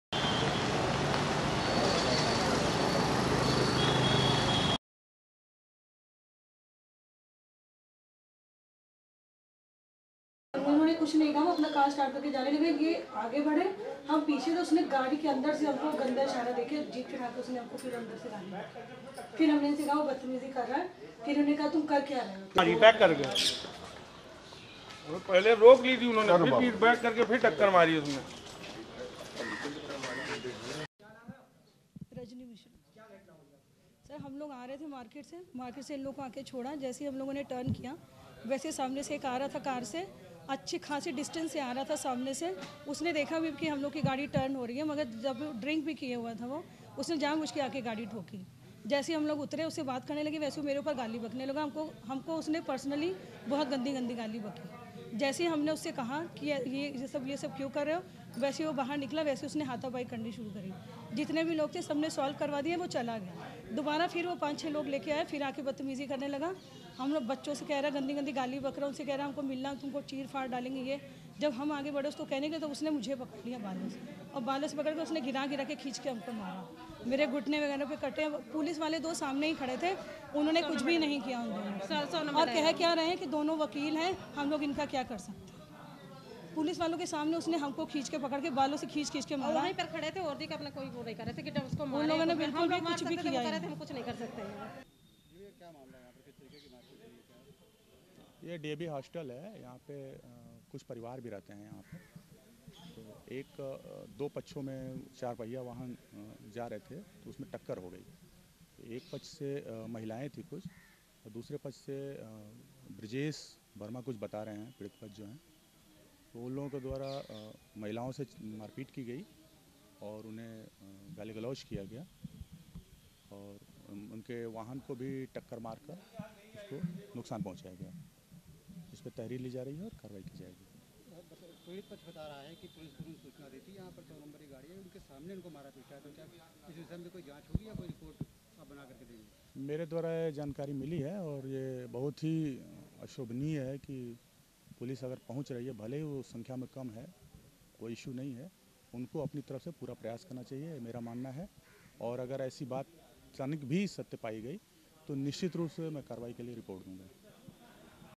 कानपुर में दबंगों ने महिलाओं को सरेआम पीटा, तमाशबीन बनी रही यूपी पुलिस
कानपुर: महानगर के सिविल लाइन पॉश इलाके में दबंगों ने सरेआम महिलाओं की पिटाई कर दी. यही नहीं महिलाओं को पुलिस के सामने भी पीटा गया, लेकिन पुलिस मुंह छिपाकर भाग गई.